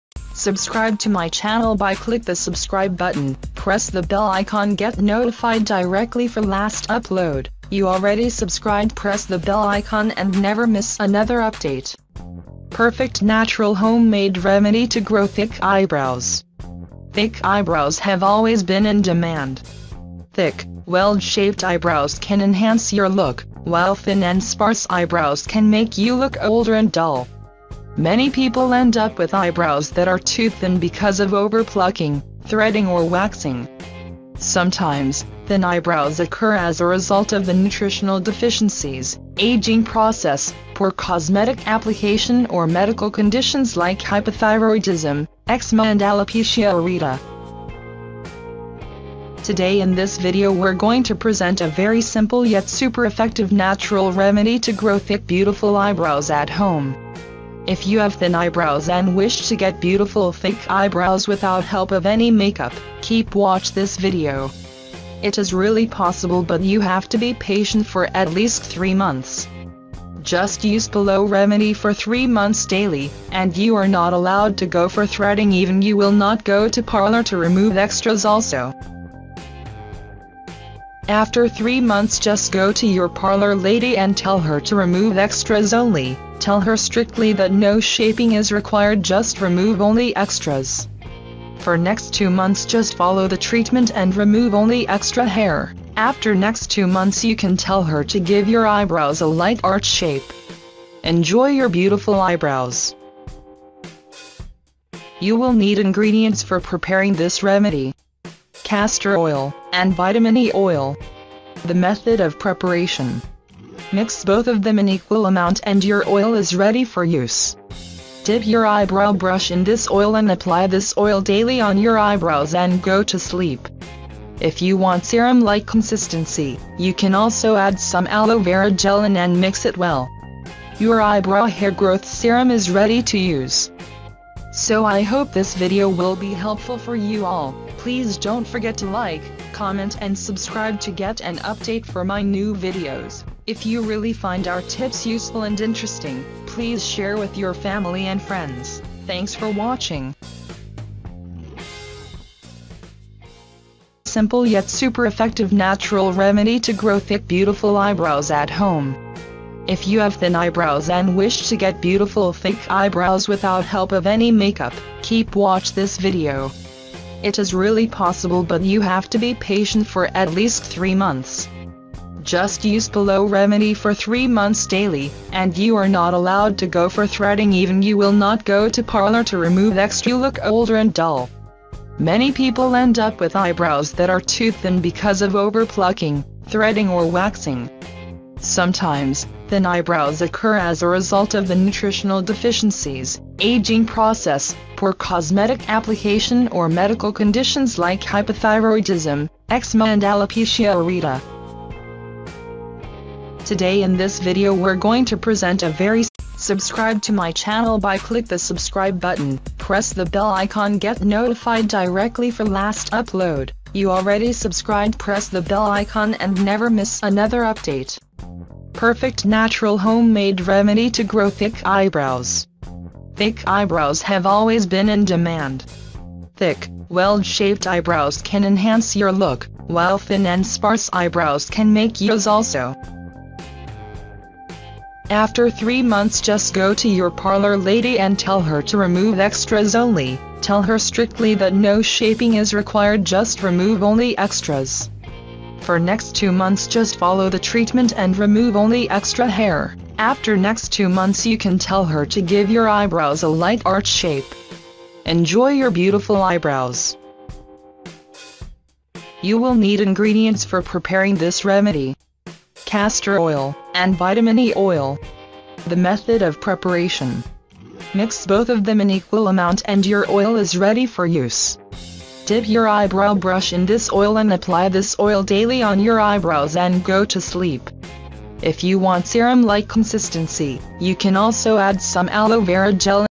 Perfect Natural Homemade Remedy To Grow Thick Eyebrows
Hello everyone. In this video we're going to present you the Perfect Natural Homemade Remedy To Grow Thick Eyebrows. Thick eyebrows have always been in demand. Thick, well-shaped eyebrows.

In this video i will show How to grow long thick eyelashes & eyebrows FAST, Guaranteed Longer and thicker eyelashes. grow eyebrows naturally, grow eyebrows naturally home remedy, grow eyebrows.